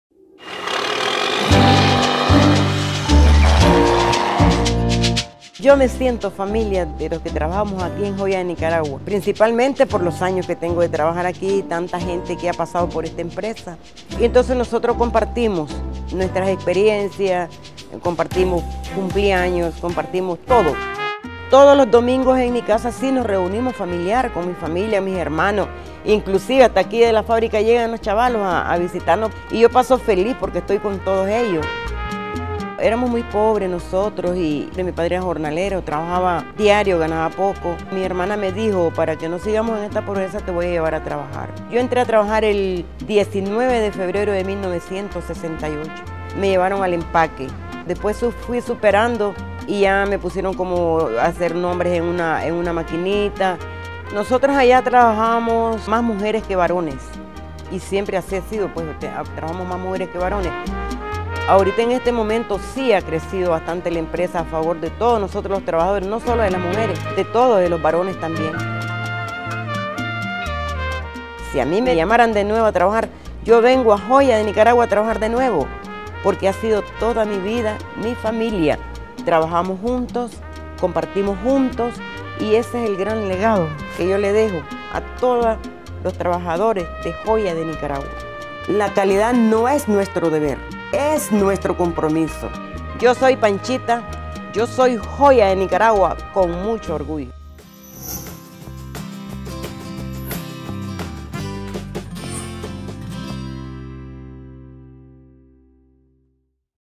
A Profile on Panchita: Joya de Nicaragua's Matriarch
The history of Joya de Nicaragua is deeply intertwined with the history of its people. Luckily, these people are still around or even working at JDN after 50 years. In the case of Panchita, for the entire time the factory has been in operation.